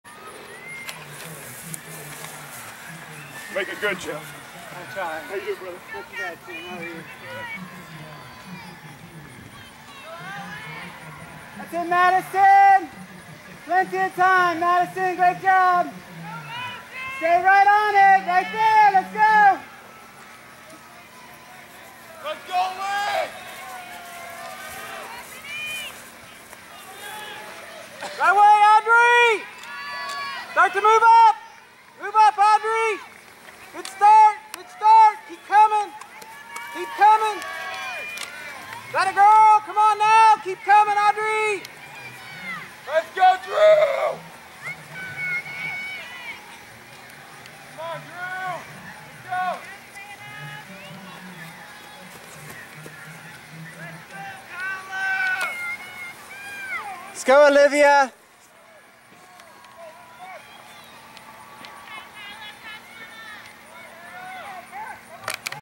Girls 5K 2
November 5, 2016 KHSAA Cross Country State Championships in Lexington, KY at the Kentucky Horse Park.